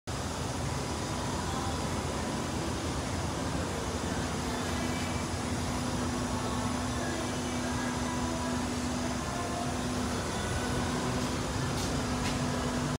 TA SERVICE TRUCK INSPECTION TAKING FOREVER
I'm a Landstar BCO aka an Owner Operator within Landstar Ranger. I have a flatbed Conestoga working everyday as a truck driver. Trucking all over the USA and I hope you like the video.